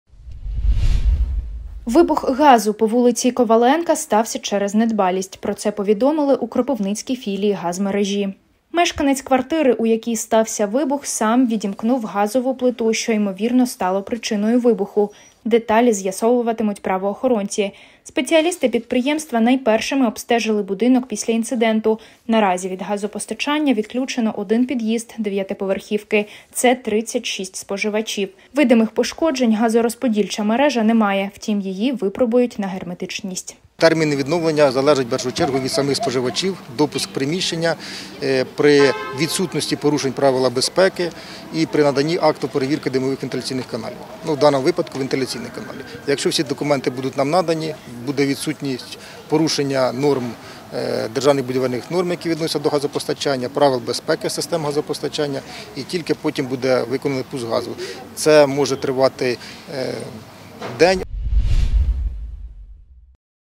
В “Газмережі” вже знають причину вибуху по вулиці Юрія Коваленка в Кропивницькому | телеканал Вітер
Вибух газу по вулиці Коваленка стався через недбалість. Про це повідомили у Кропивницькій філії “Газмережі”. Мешканець квартири, у якій стався вибух сам відімкнув газову плиту, що ймовірно стало причиною вибуху. Деталі з'ясовуватимуть правоохоронці. Спеціалісти підприємства найпершими обстежили будинок після інциденту. Наразі від газопостачання відключено один під'їзд дев'ятиповерхівки. Це 36 споживачів. Видимих пошкоджень газорозподільна мережа  немає, втім її випробують на герметичність.